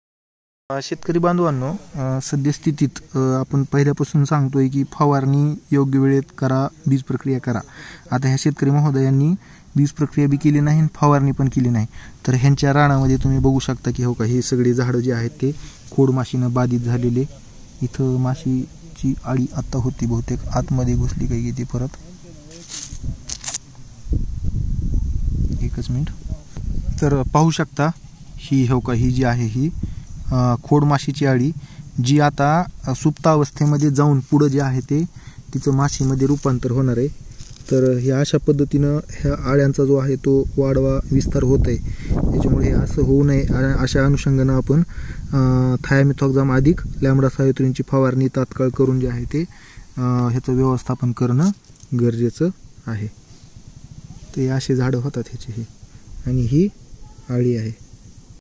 सोयाबीन पिकामधील खोडमाशी प्रादुर्भाव व्यवस्थापन
सोयाबीन पिकामधील खोडमाशी प्रादुर्भाव लोकेशन धसवाडी तालुका अंबाजोगाई सोयाबीन व्हेरायटी केडीएस ७२६ संबंधित शेतकरी महोदयांनी शिफारशीतील कीडनाशकाची बीजप्रक्रिया केली नाही तसेच प्रतिबंधात्मक फवारणी सुद्धा घेतली नाही पेरणी होऊन एक महिना झाला